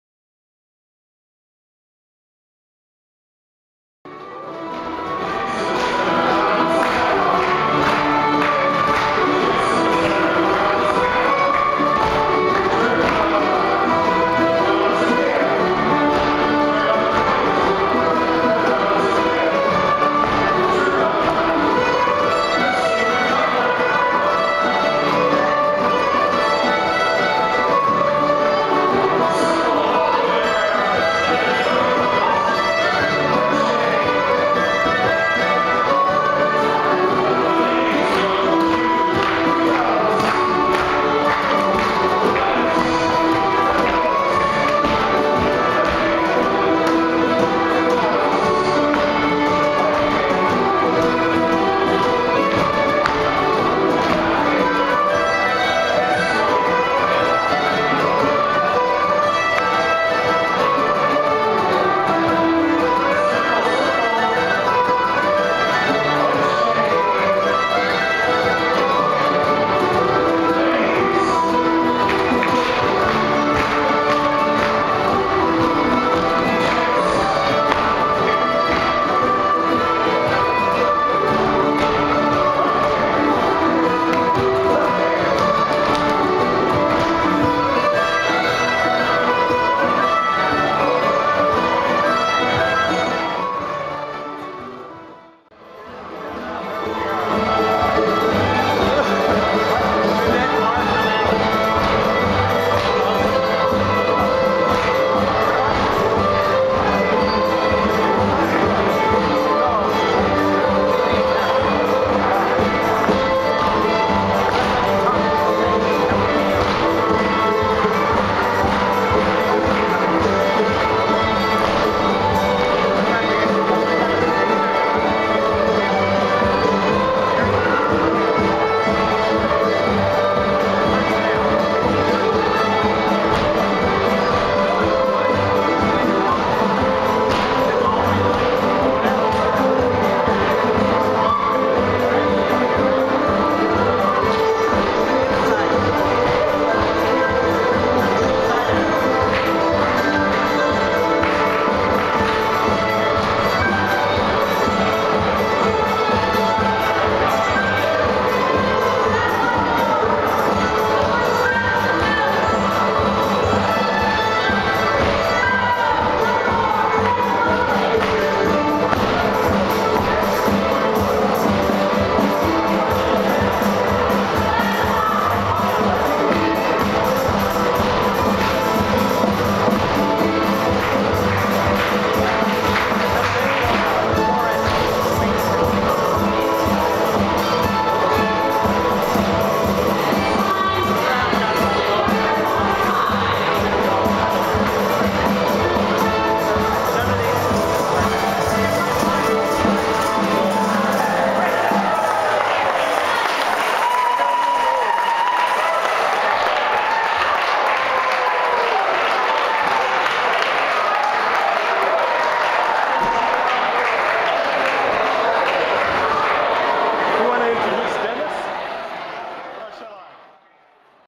OFB Contra Jam - Dancing Clips - 2010-12
Some dancing to the music of the big band OFB contra jam.  Laura Lengnick and Art Shuster leading the band.